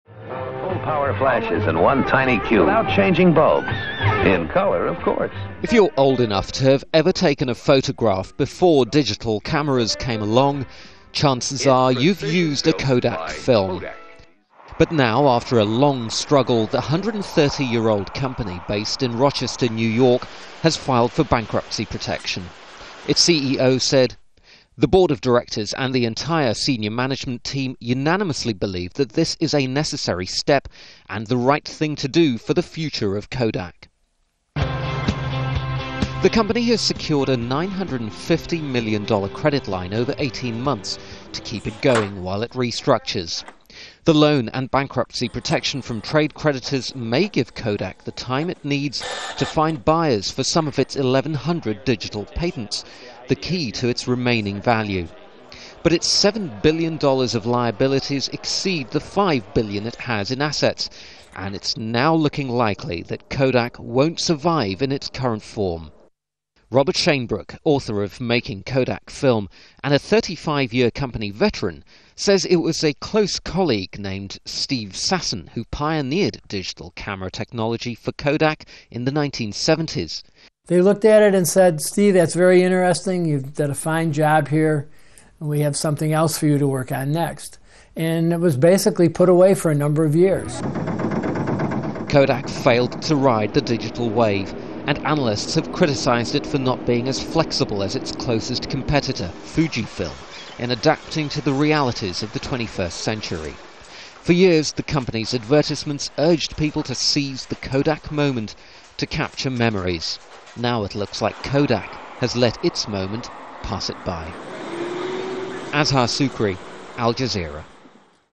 Kodak goes bankrupt amid digital world of film
Eastman Kodak, the US-based company which brought photography to the masses over a century ago, has filed for bankruptcy protection after years of failing to become a profitable seller of modern consumer products.

The 130-year-old photographic film pioneer said it had also obtained a $950m loan from Citigroup, the US financial firm, to help keep it going.

Al Jazeera's Azhar Sukri reports.